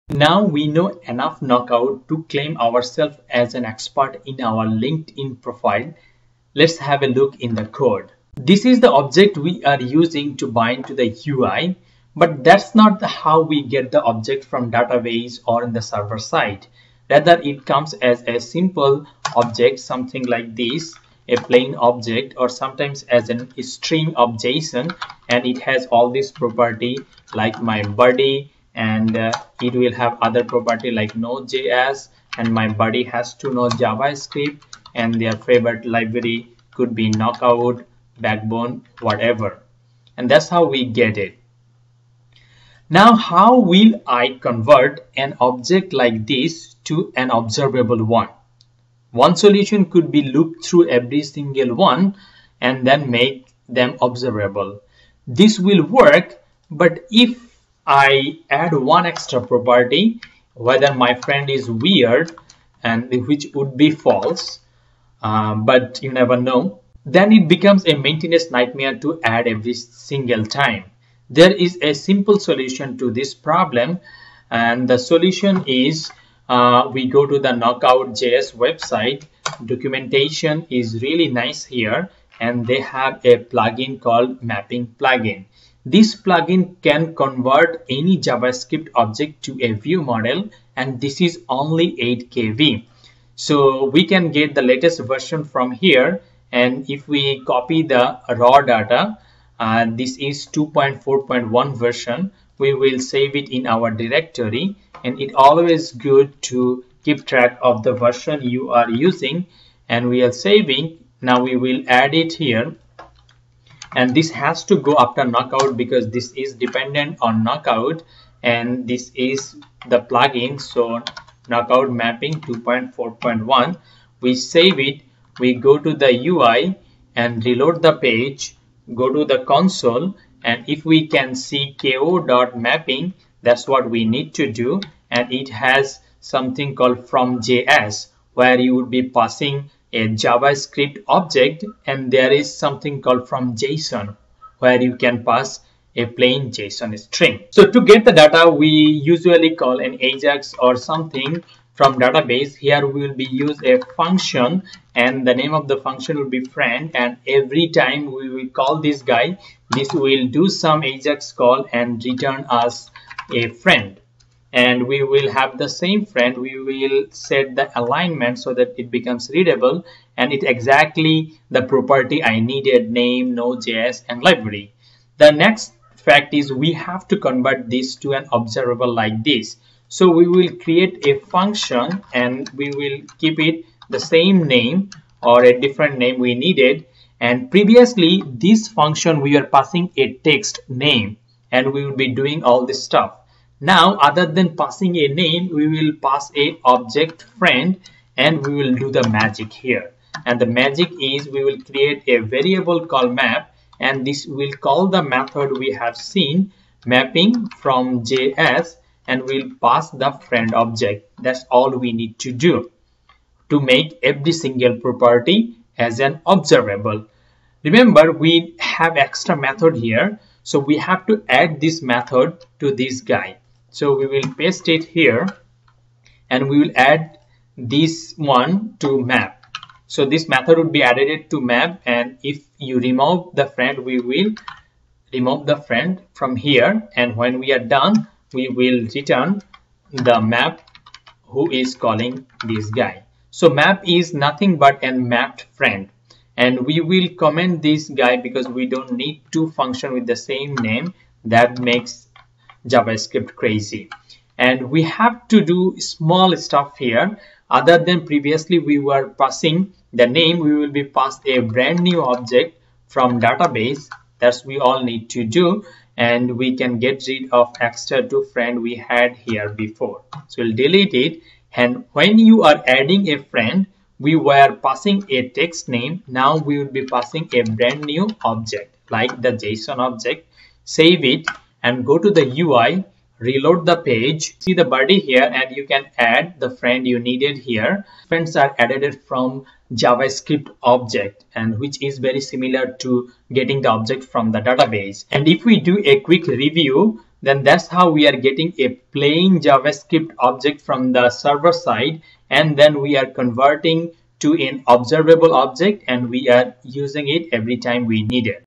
KnockoutJS The Basic for Begginer Part-9 Mapping Plugin work with real data
Mapping Plugin is a very light weight (8kb) but makes your life super easy. It can convers object or json to an observable object. Also comes with tons of other feature. In this video, i show how u convert data from server side without changing functionality